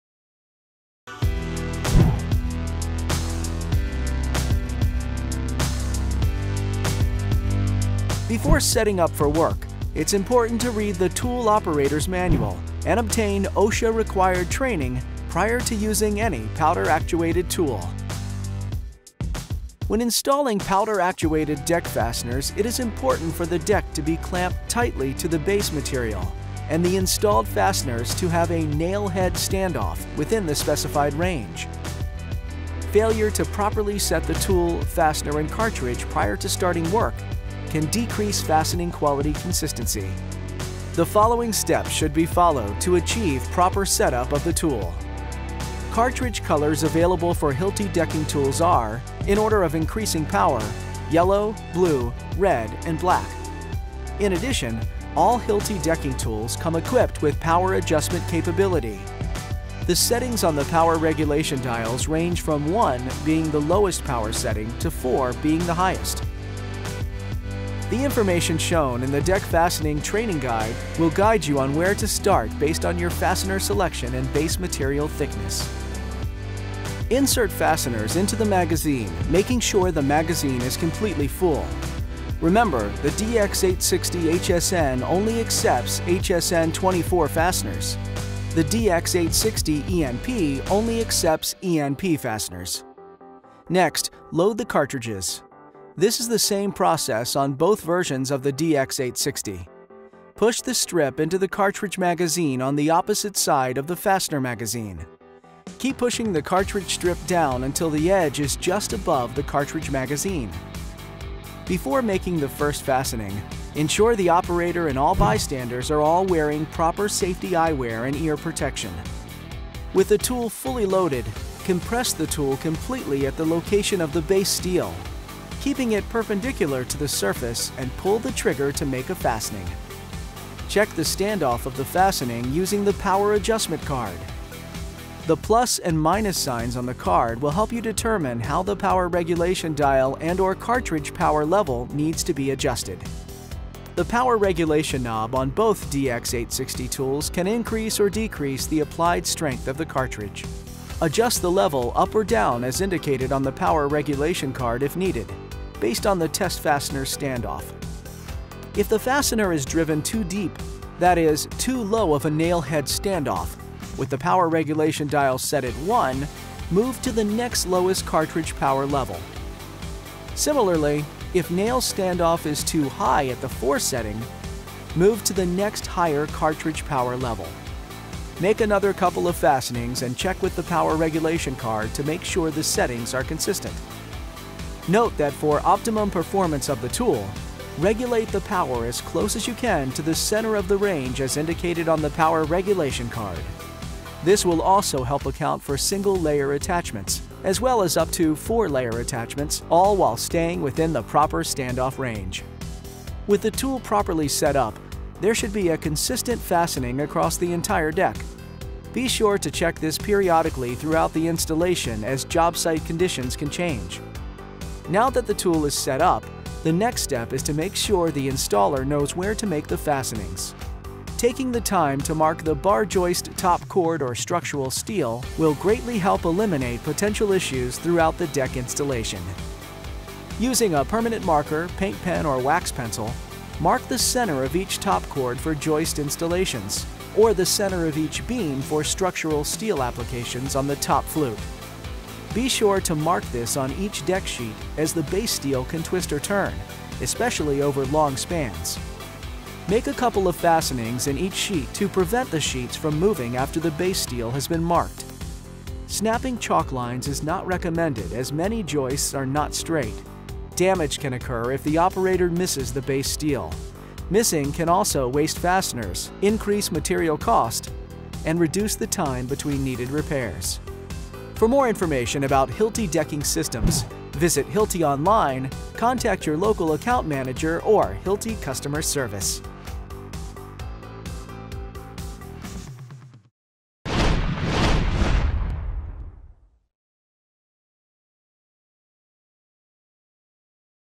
DEMONSTRATION of jobsite and tool setup of the Hilti powder-actuated DX 860
In this video, you'll learn how to setup your Hilti DX 860 and jobsite for optimal performance.